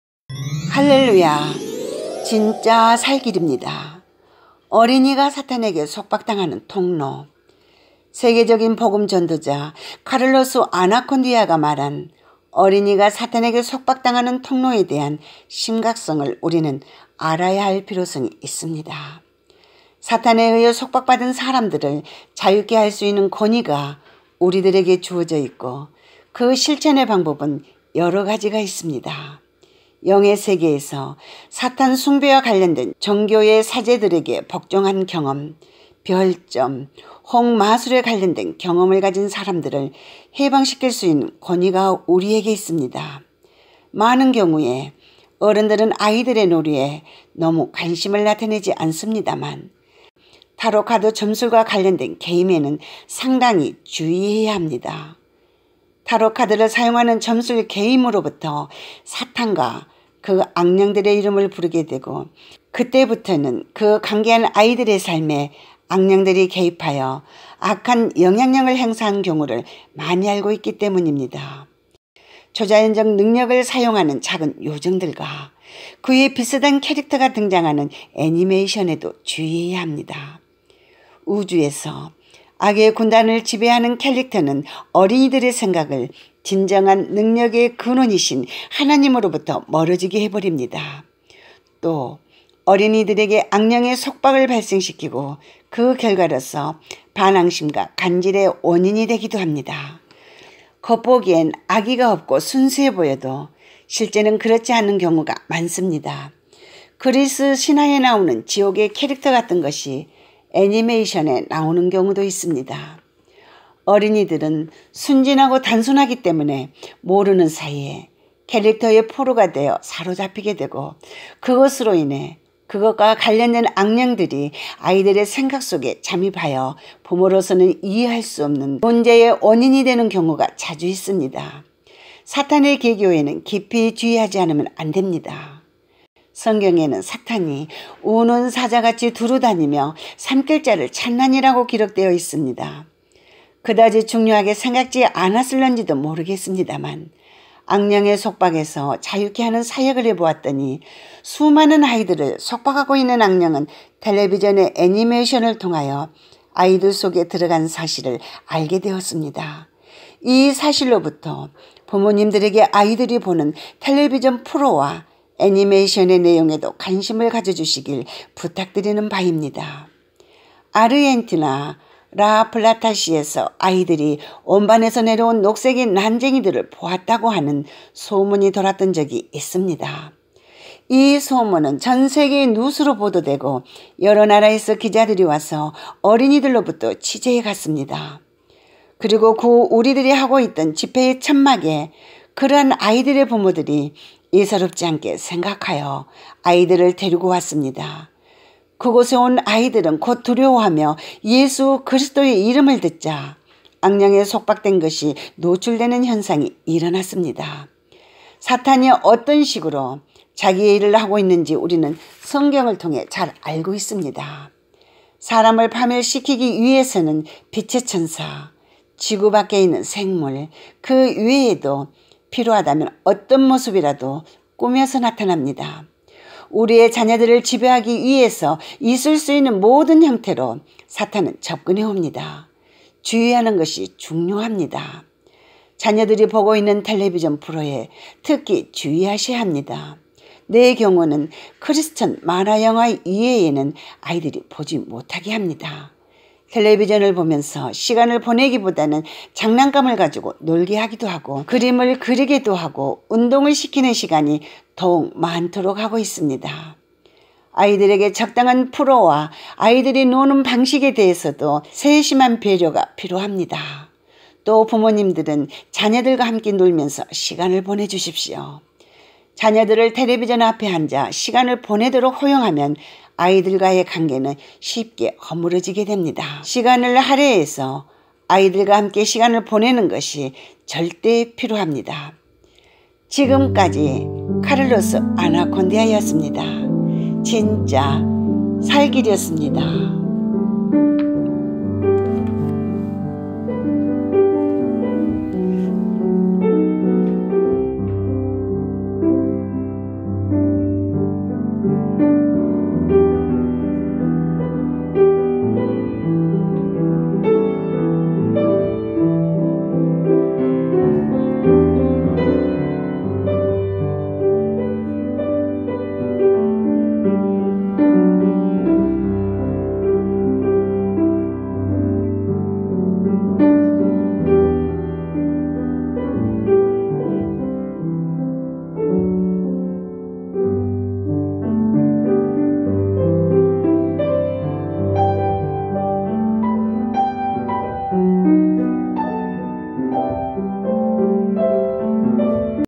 악령이 어린이들에게 들어가는 통로 (카를로스 아나콘디아
세계적인 복음 전도자 카를로스 아나콘디아 가 어린이가 사탄에게 속박당하는 통로가 별점, 마술, 카드점술, 타롯카드 점술게임, 애니메이션, 만화영화, TV 라는 사실을 사역을 하면서 알게 되었다.
부모님들이 이 사실을 반드시 알아야 할 필요성에 대해서 강력히 말하고있다.